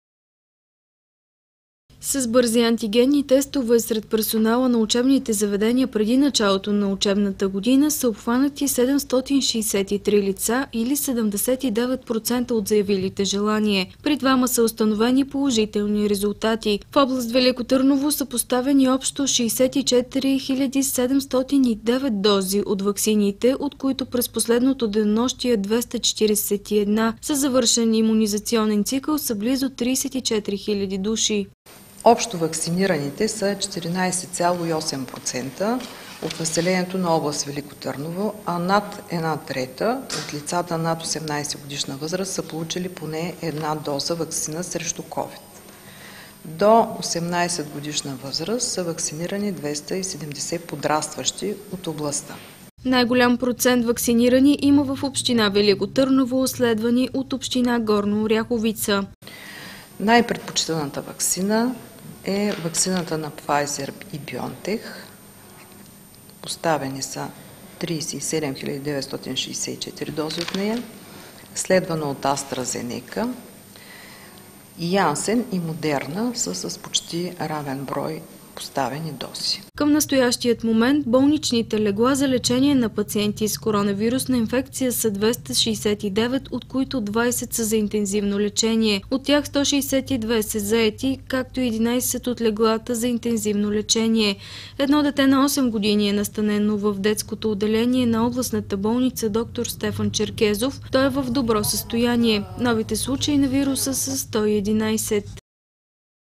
Ученици от 10 училища в областта са преминали на дистанционно обучение заради случаи на Covid-19
Ученици от 10 училища в областта са преминали на дистанционно обучение заради регистрирани случаи на Covid-19. Това стана ясно на заседание на Областния кризисен щаб за борба с коронавирус. Грамота от името на министъра на здравеопазването получи и общопрактикуващият лекар д-р Пламен Латев от Павликени. От началото на ваксинационната кампания той е имунизирал 2 363 лица.
С бързи антигенни тестове сред персонала на учебните заведения, преди началото на учебната година, са обхванати 763 лица, или 79% от заявилите желание. При двама са установени положителни резултати.
В област Велико Търново са поставени общо 64 709 дози ваксини, от които през последното денонощие – 241. Със завършен имунизационен цикъл са близо 34 000 лица.
 д-р Евгения Недева, директор на РЗИ Велико Търново
 “Общо ваксинираните са 14,8% от населението на област Велико Търново. Над 1/3 от лицата  над 18-годишна възраст са получили поне една доза ваксина срещу Ковид. До 18-годишна възраст са ваксинирани 270 подрастващи от областта.”
Най-голям процент ваксинирани има в Община Велико Търново, следвани от Община Горна Оряховица.
  “Най-предпочитаната ваксина е ваксината на Пфайзер. Поставени са 37 964 дози от нея, следвана от Астра Зенека. Янсен и Модерна са с почти равен брой поставени дози.”
Към настоящия момент болничните легла за лечение на пациенти с коронавирусна инфекция са 269, от които 20 са за интензивно лечение. От тях 162 са заети, както и 11 от леглата за интензивно лечение. Едно дете на 8 години е настанено в детското отделение на Областната болница „Д-р Стефан Черкезов“ и е в добро състояние.
Новите случаи на вируса са 111.